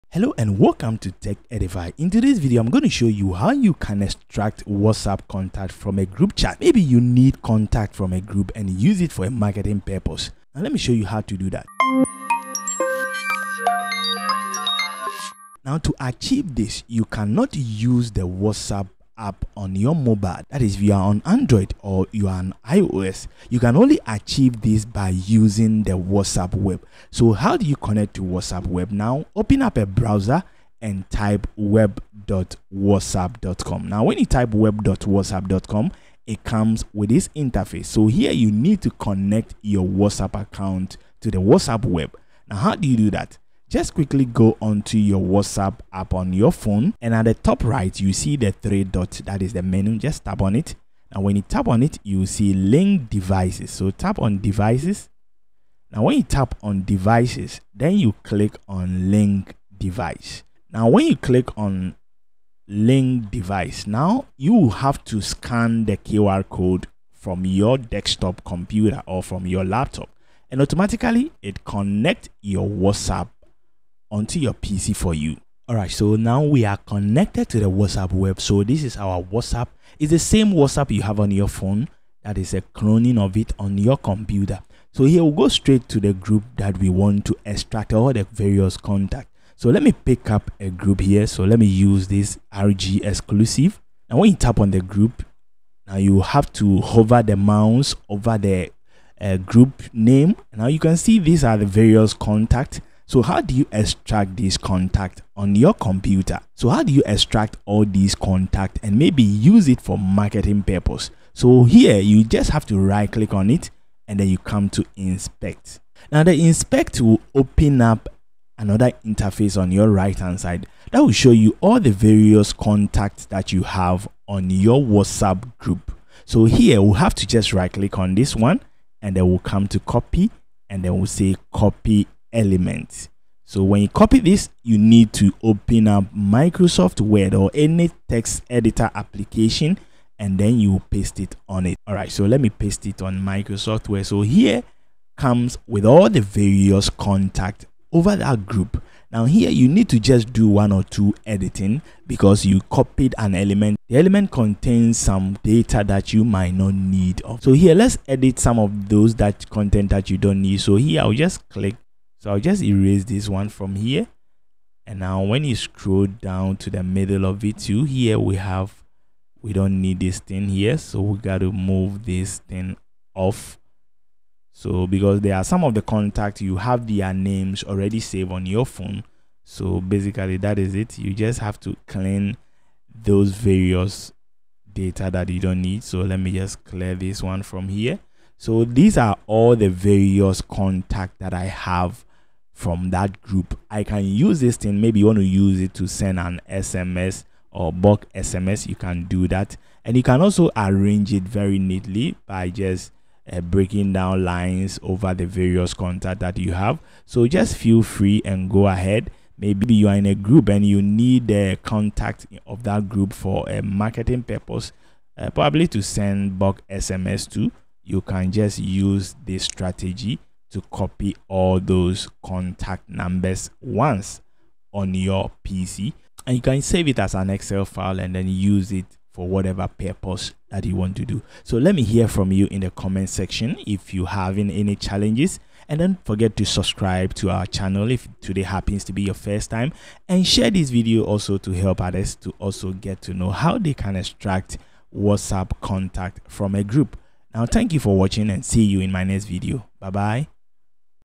How to Export All WhatsApp Group Contacts-Copy All with Ease
DOWNLOAD THE DIGITAL INCOME TOOLKIT-A Toolkit with About 15 Legit Ways to help you generate money using the internet.

Learn how to export all WhatsApp group contacts easily in this step-by-step tutorial. Whether you're managing a business group or organizing contacts for personal use, this guide will show you the most efficient way to download and save group members' details. Perfect for marketers, business owners, and anyone who needs to manage contacts effectively!